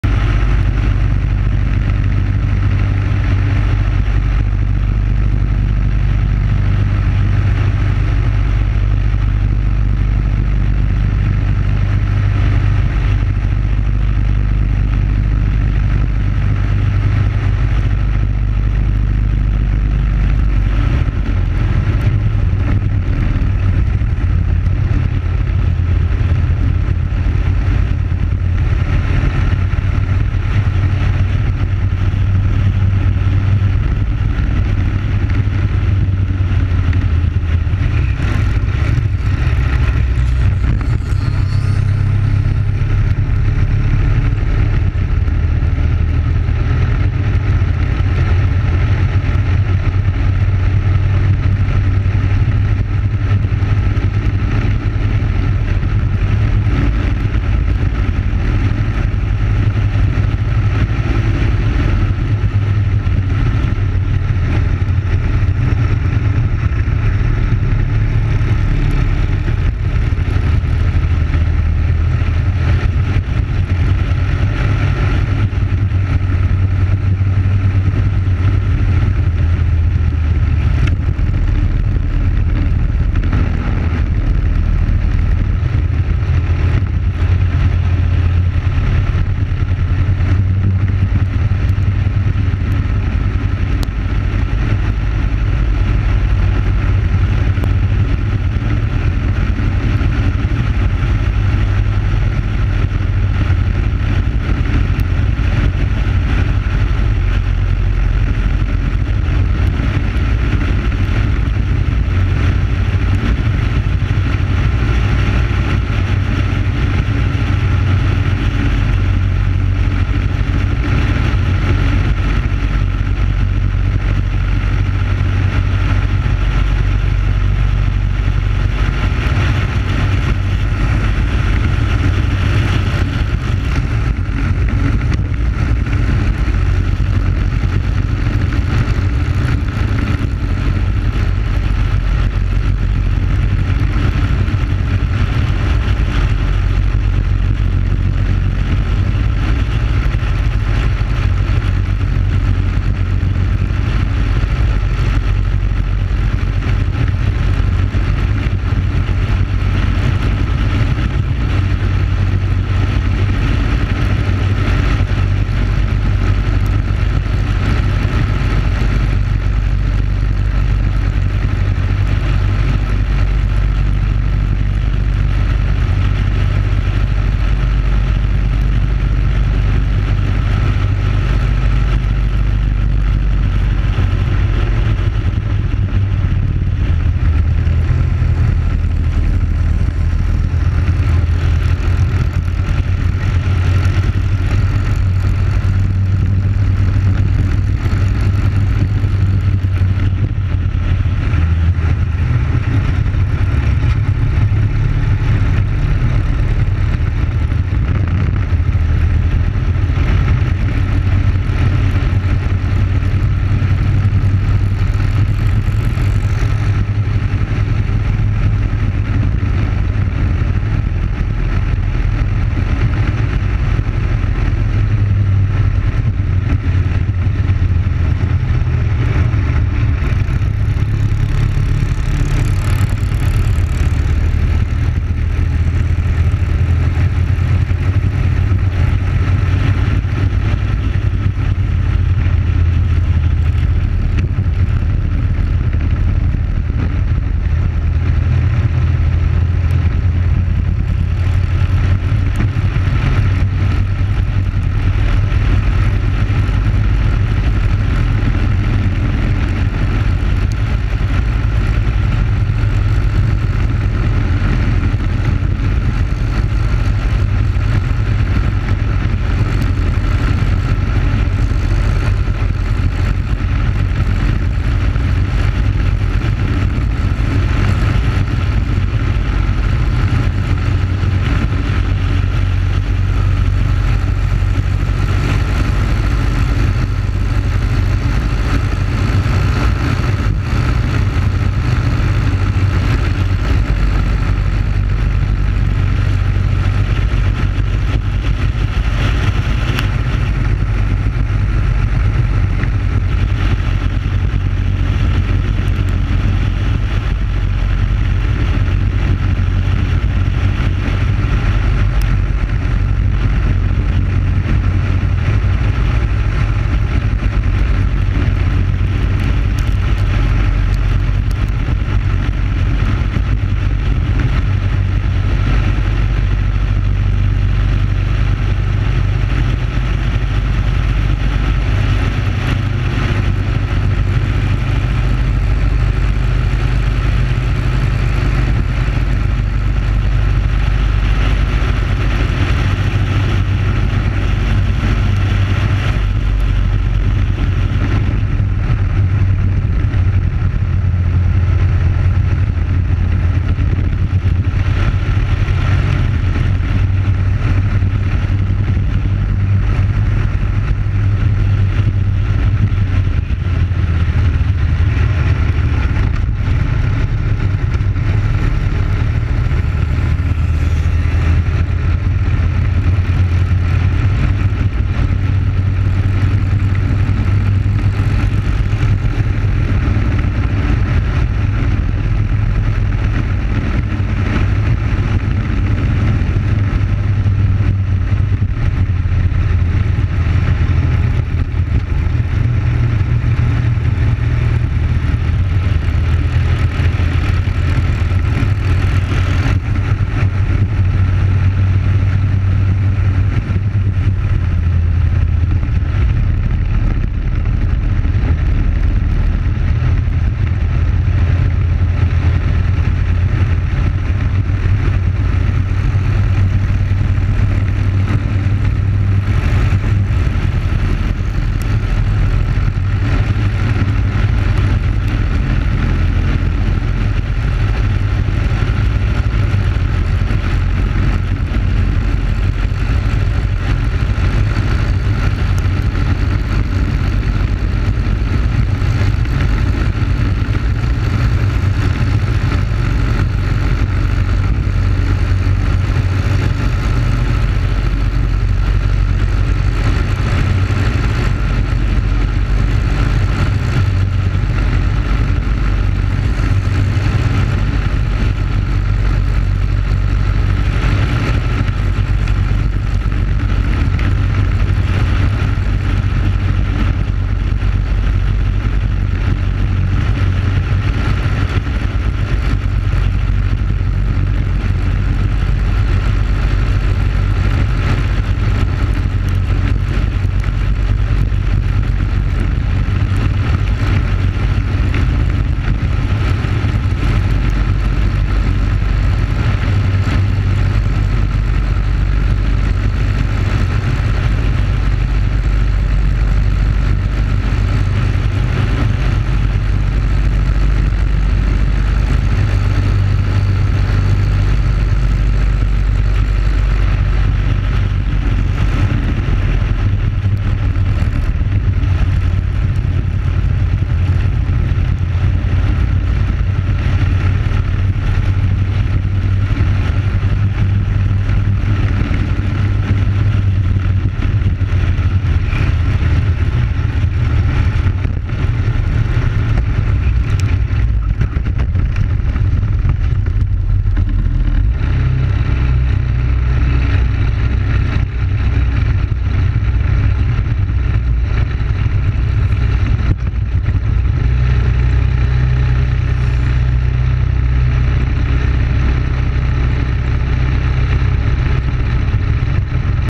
Daytona Briggs Animal 360 Battle
A good battle in the Animal 360 class ending with a spin and hit in the infield. Daytona 2013 WKA Road Racing